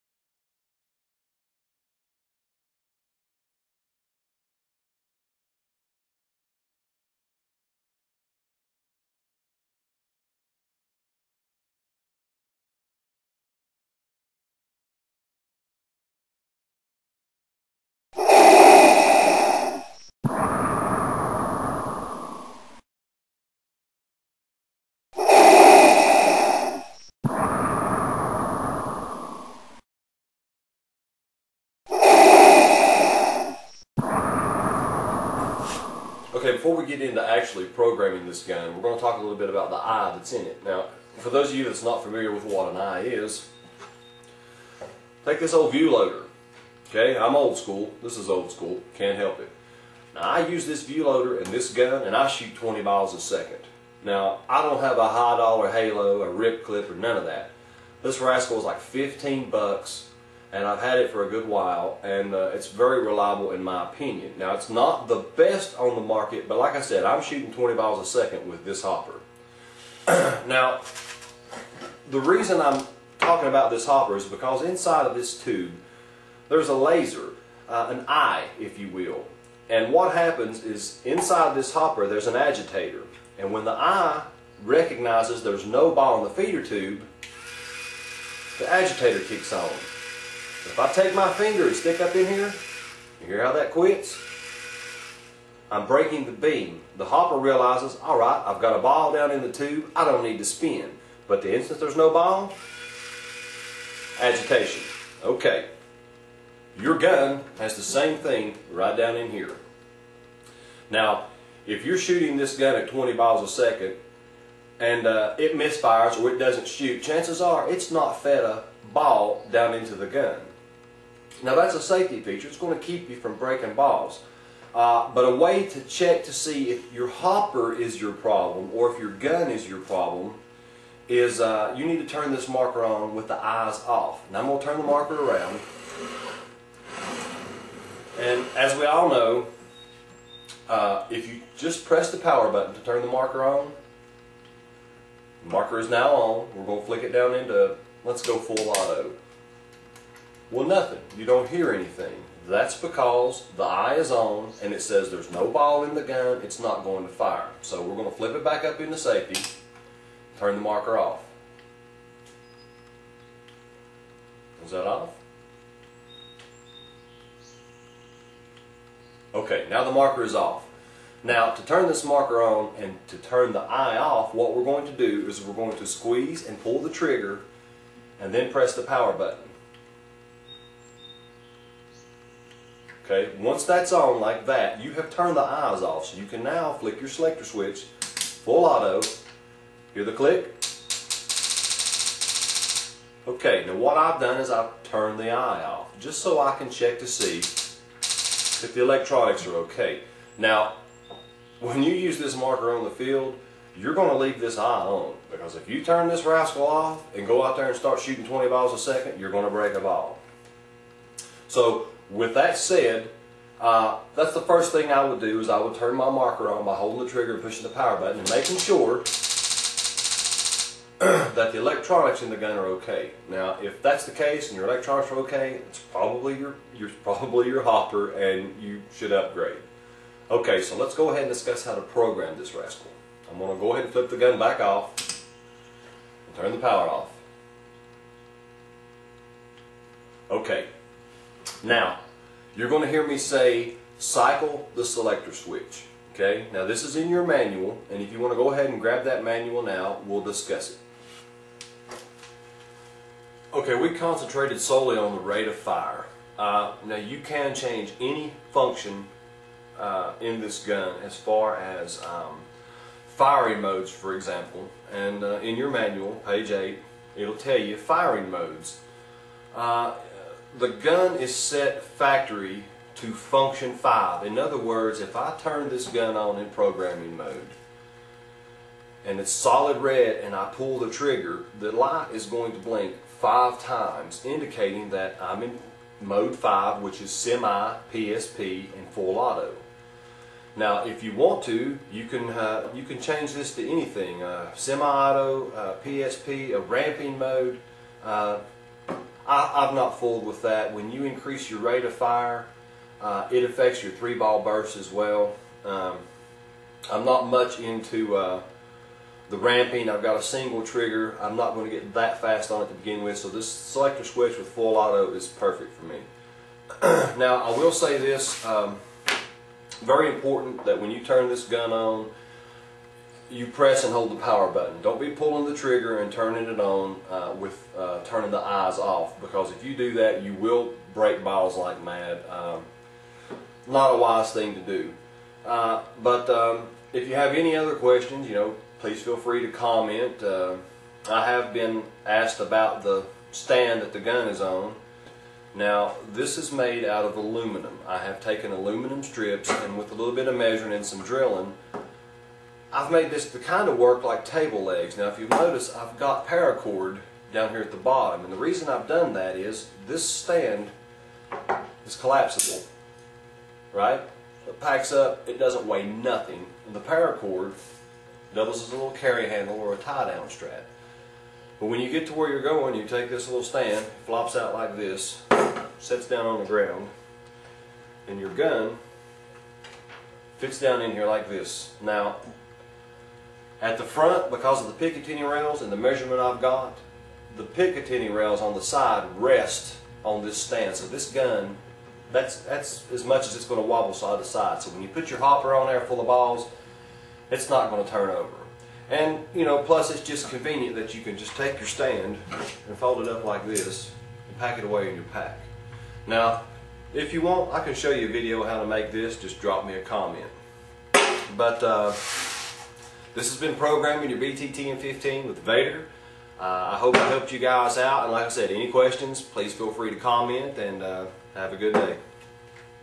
The eyes of the BT TM 15
This was edited out of the video "how to program the BT TM 15". This section deals with checking the electronics in the marker, more specifically the eyes system.